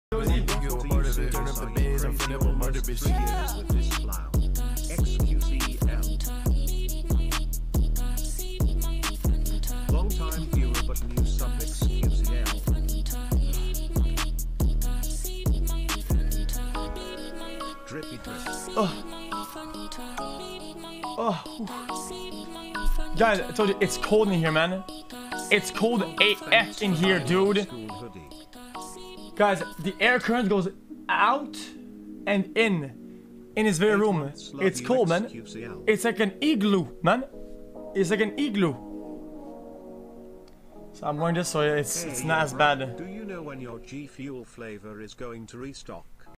xQc wearing his cozy hoodie for his igloo
e glue

clips from twitch